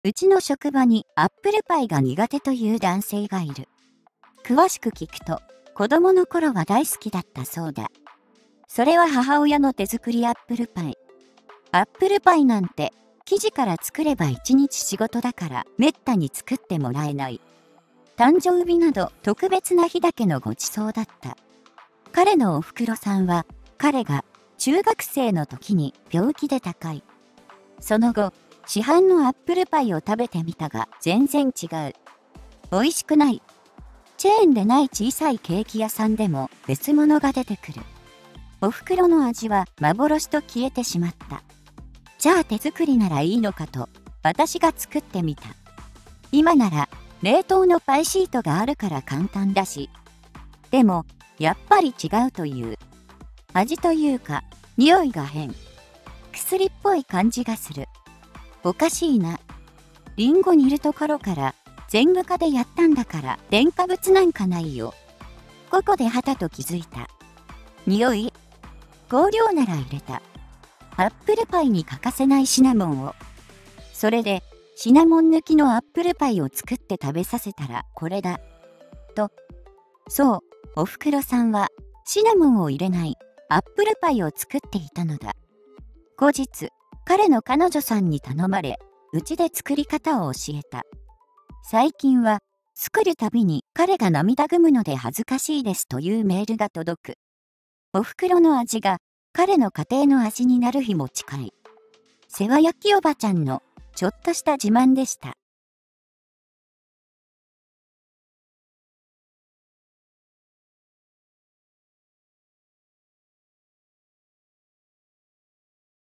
【おふくろの味】同僚男「アップルパイが子供のころは大好きだったけど、今は苦手」私（ もしかして…？）【痛快・スカッとジャパン】
※動画音楽
　LAMP BGM　様